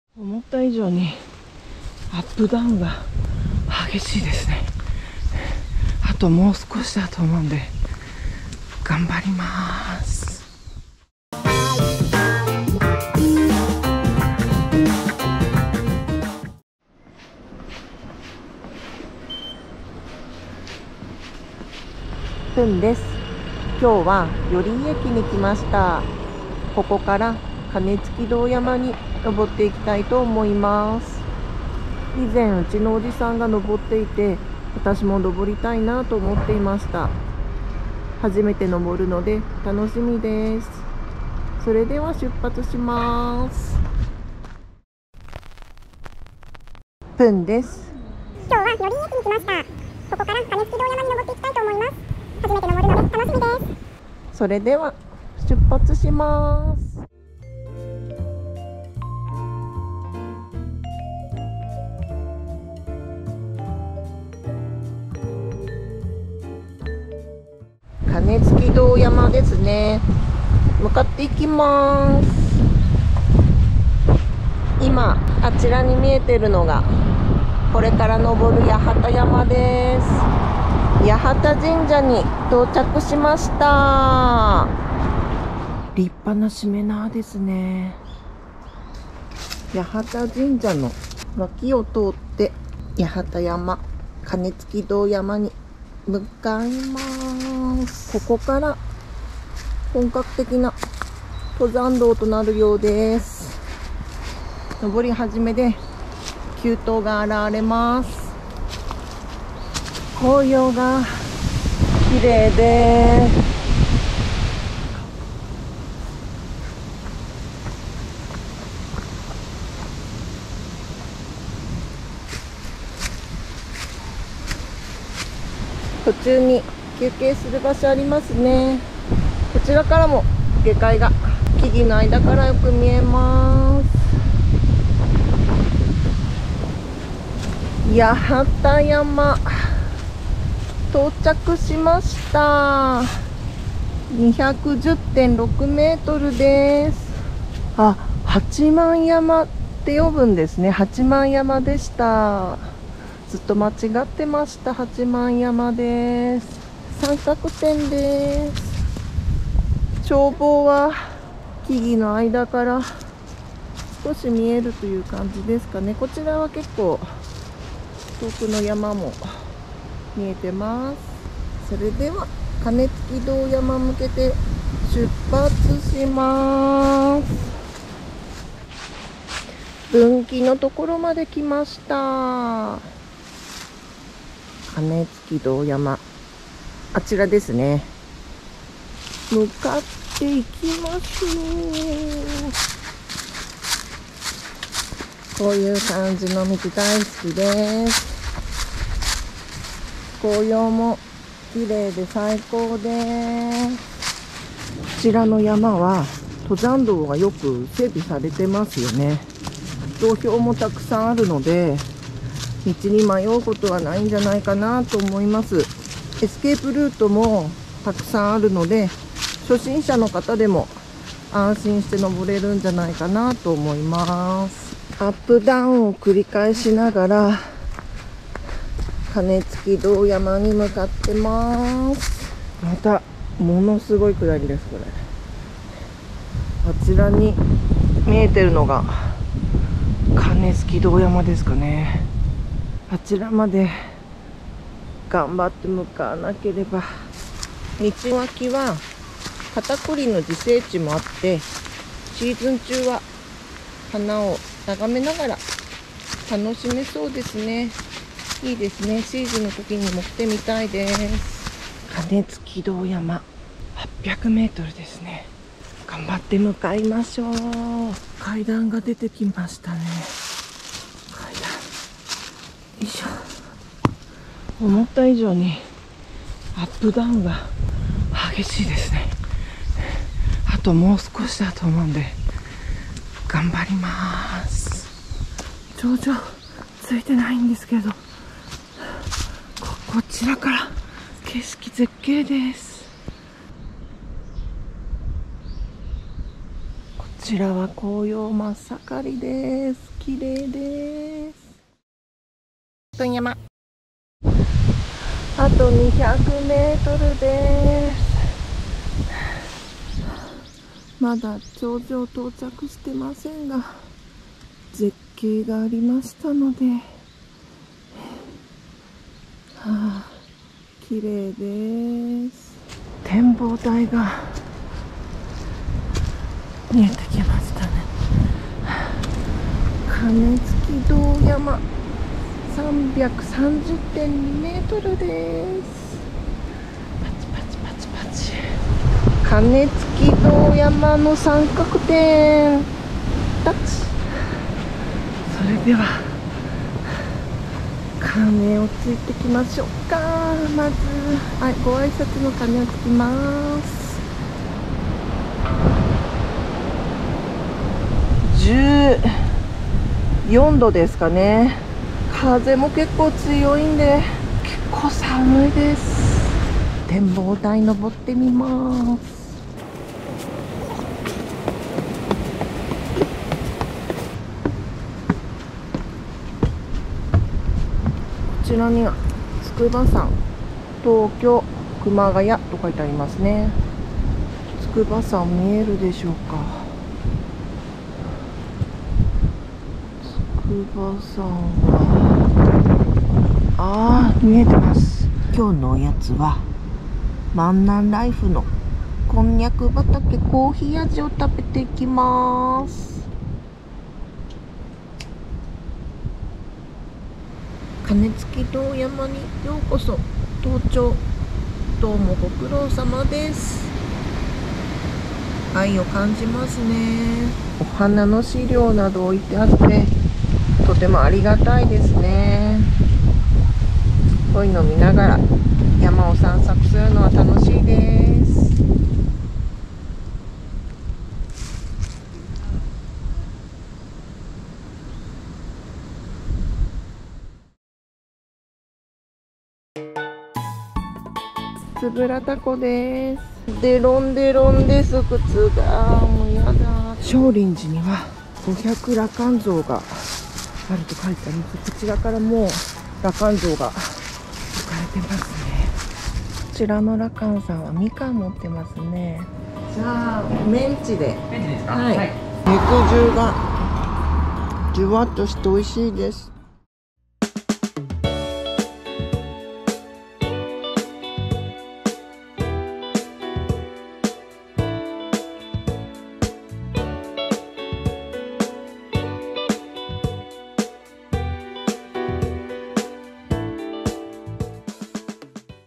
鐘撞堂山周辺を歩く旅(前編)
妻ぷんさんが鐘撞堂山を散策しました
今回はその動画の前編となります
 寄居駅 → 八幡山 → 鐘撞堂山 → 円良田湖 →
 少林寺 (羅漢山) → 秩父屋(お肉屋) → 玉淀駅
と歩いたようです
ぷんさんは初めての鐘撞堂山
前から行くのを楽しみにしていました